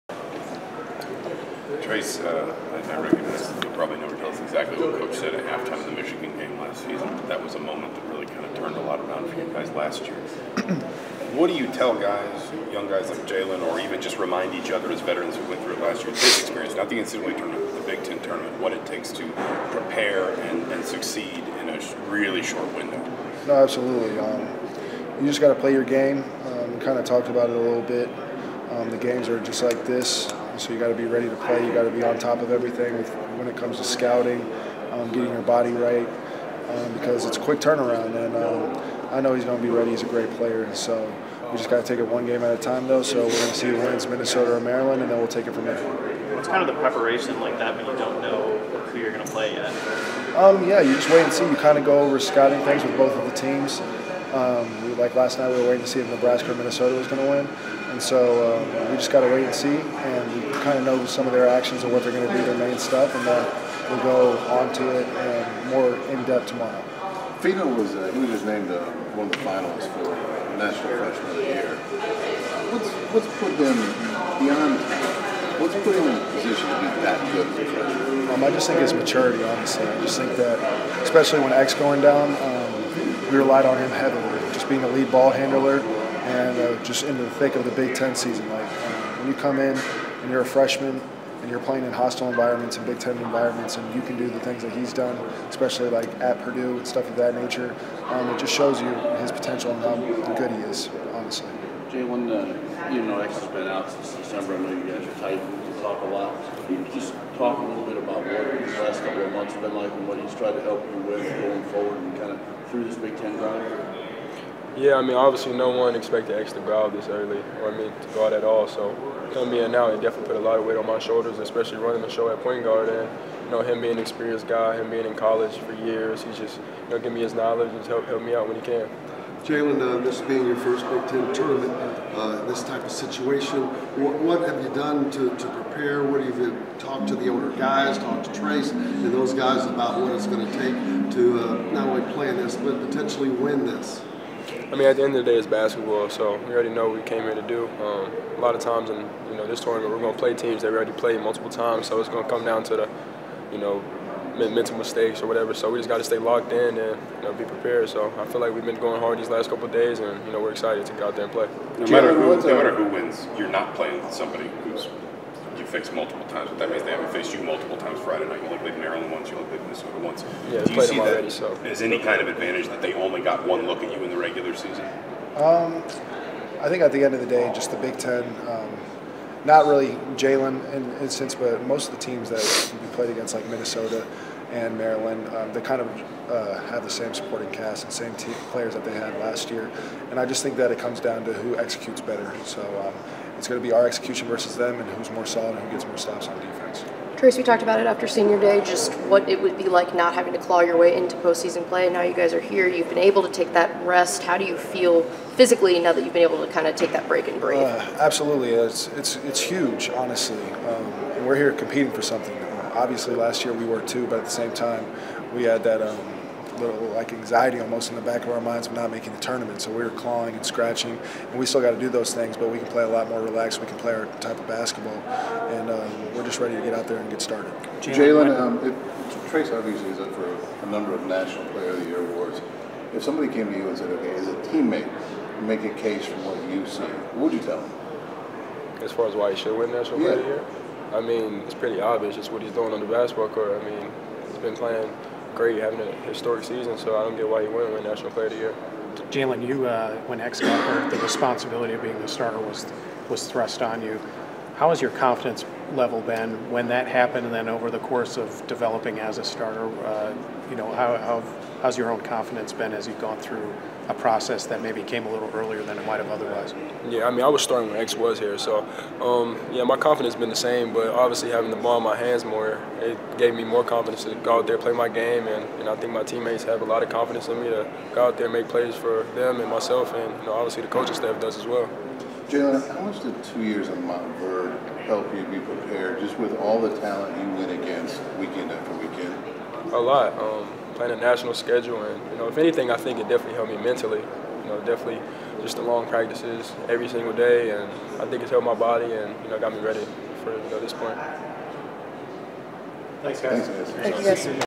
Trayce Jackson-Davis, Jalen Hood-Schifino media availability: March 9, 2023
Trayce Jackson-Davis and Jalen Hood-Schifino addressed the media on Thursday, March 9 in advance of Indiana's Big Ten tournament quarterfinal game. (Video courtesy of IU Athletics.)